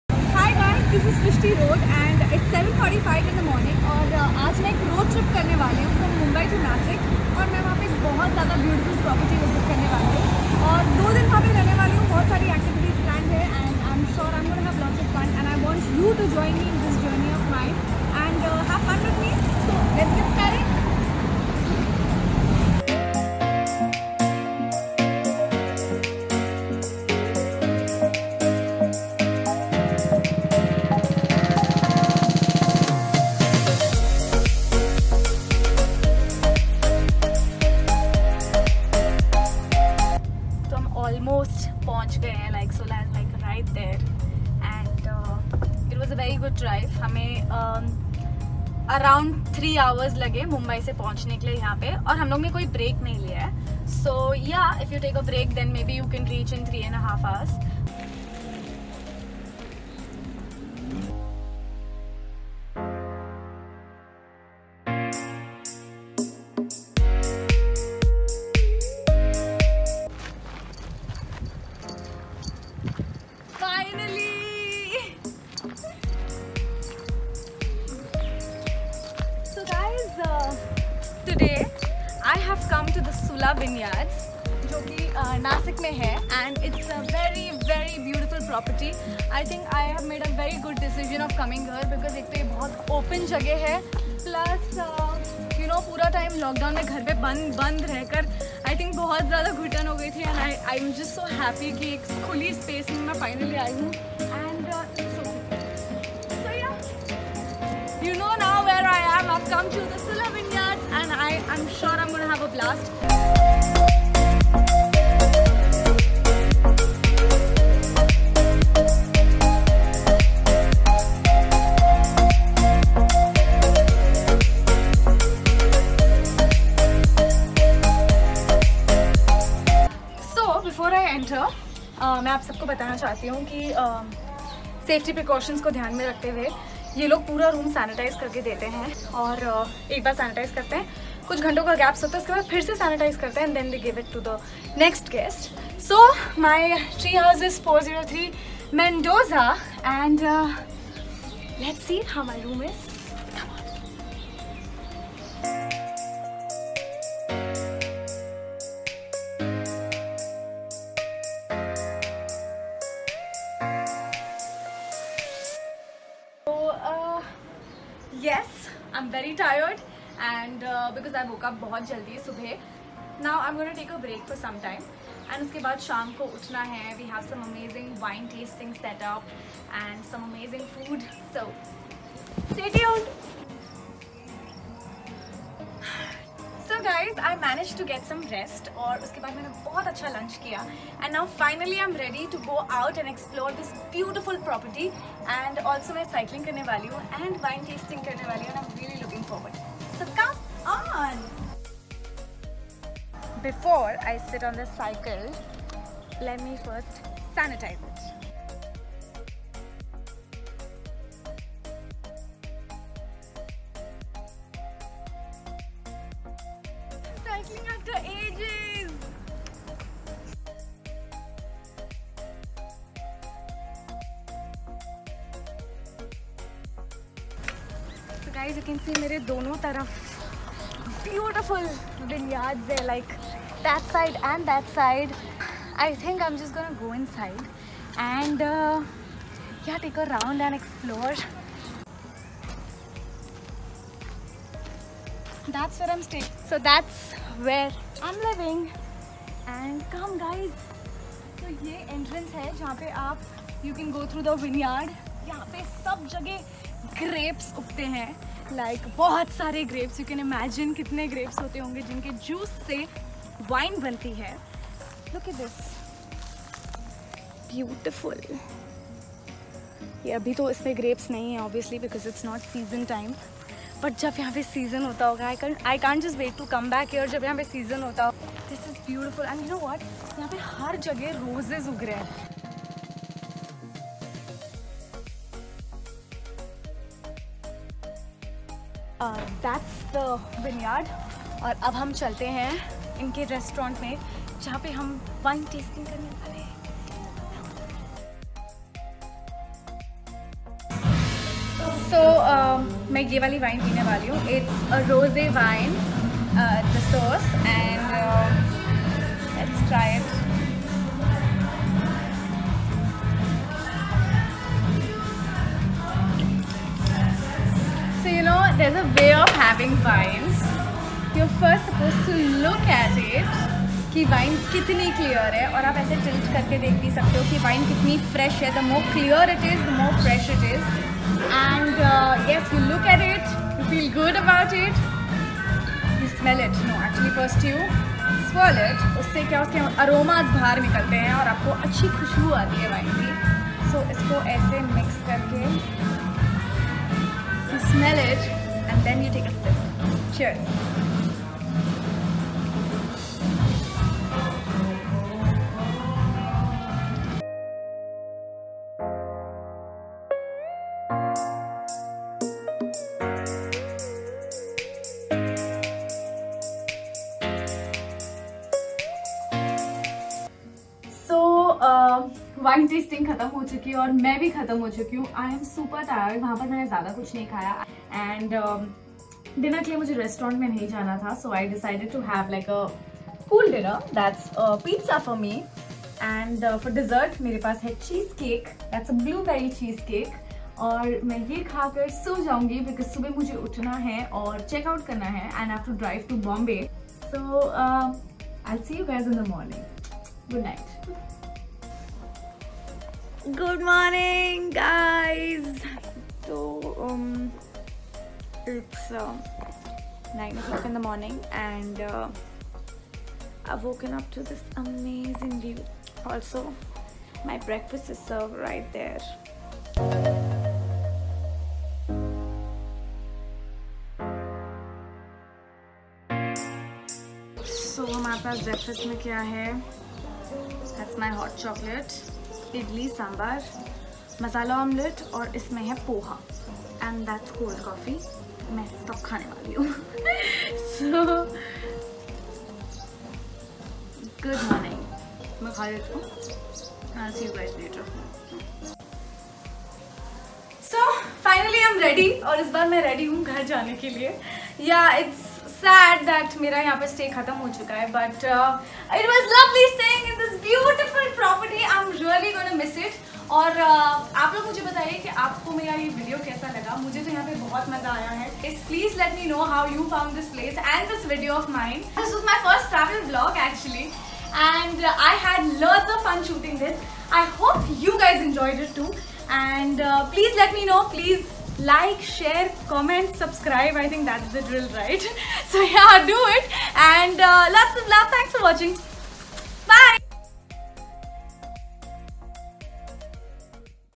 First Road Trip After Covid l Travel With Srishty Rode To The Vineyards
The pandemic left us in doors and took away so much that we were used to otherwise.  Travel was one of those that all of us were missing. I finally gathered the courage to do my first trip recently to Sula Vineyards and what an amazing experience it was.  Here I am sharing my travel journey with you as my first youtube video and hoping to connect with all of you as well as entertain you in a different way.